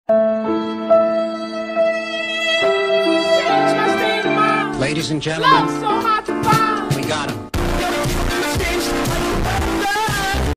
RTX horizontally spinning car
Bad Car to Good Car
RTX OFF to RTX ON
Vdoc to Vdoc

low quaility horizontally spinning low poly car VS RTX horizontally spinning McLaren rendered with true to life lighting

This is the wildest viewing of both sides of the RTX spectrum!

This is a spinning car meme. Horizontally spinning car, spinning car, car meme, car, meme, funny, silly. RTX version

Let's be honest, the car we all want was the blue one.